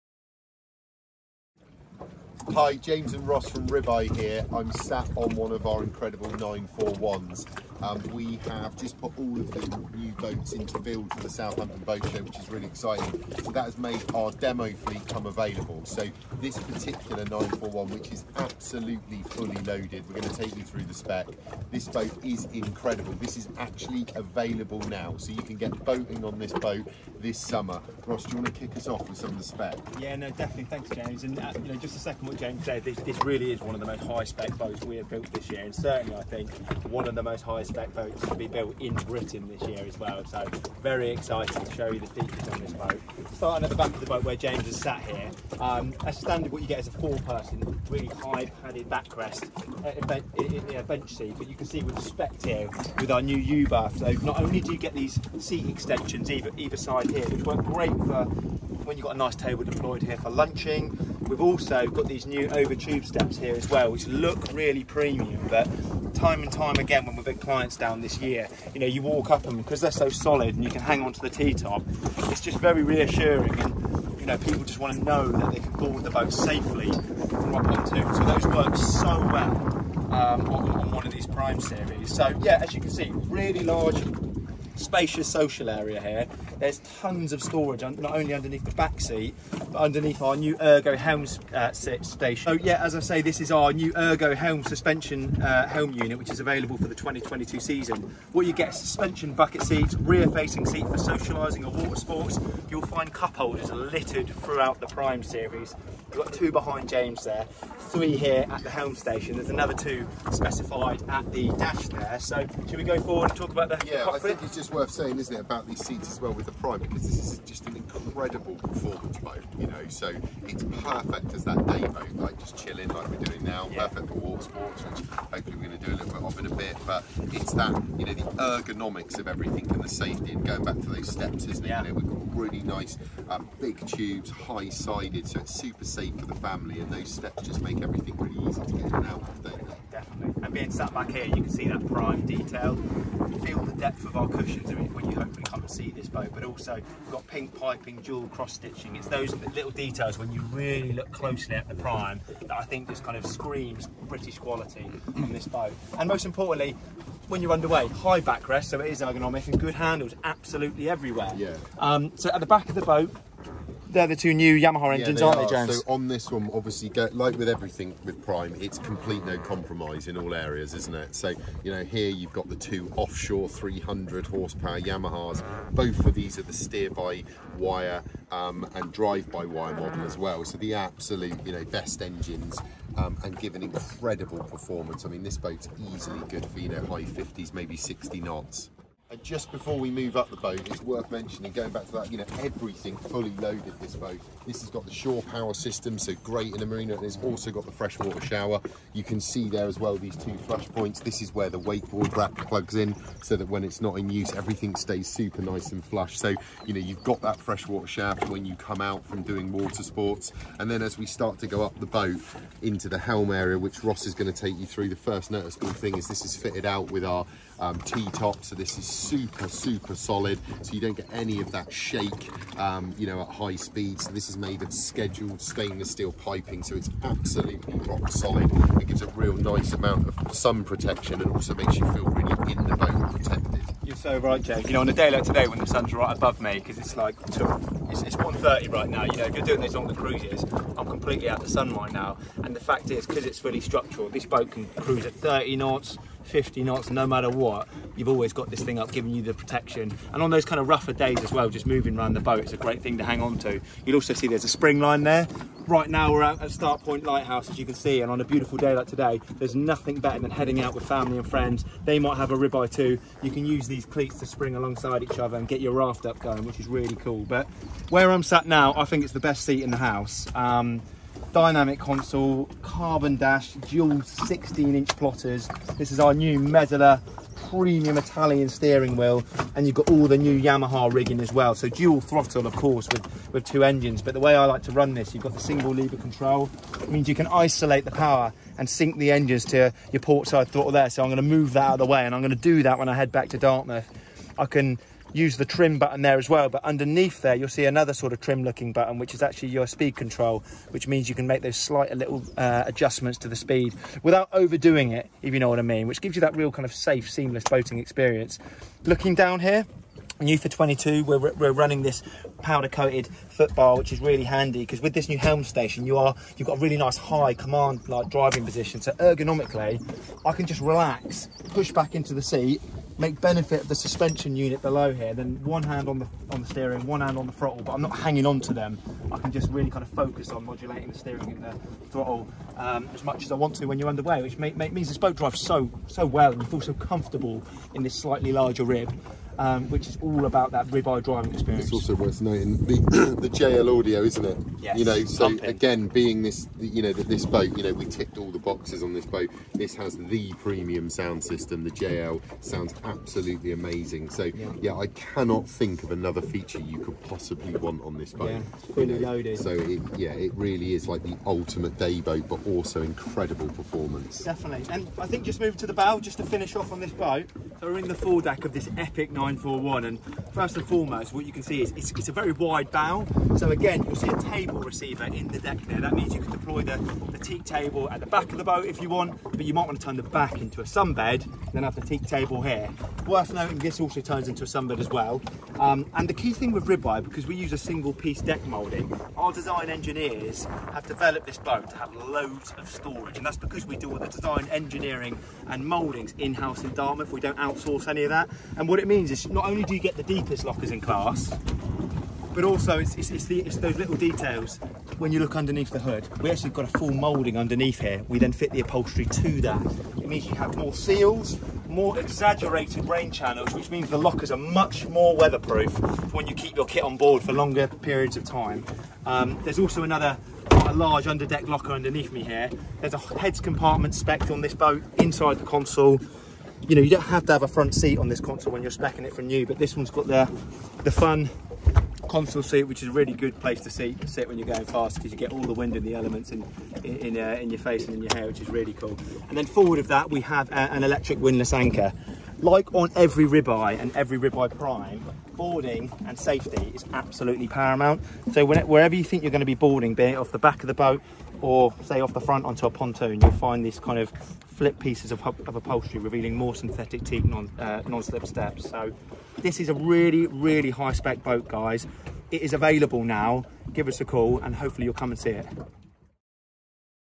Ribeye 2022 PRIME NINE41 Demo Boat FOR SALE!
NEW 2022 NINE41 – AVAILABLE  NOW!

The Ribeye Boats team is thrilled to present a new build PRIME NINE41 that is currently having its final finishing touches applied.

For further details on the best RIB in Britain get in touch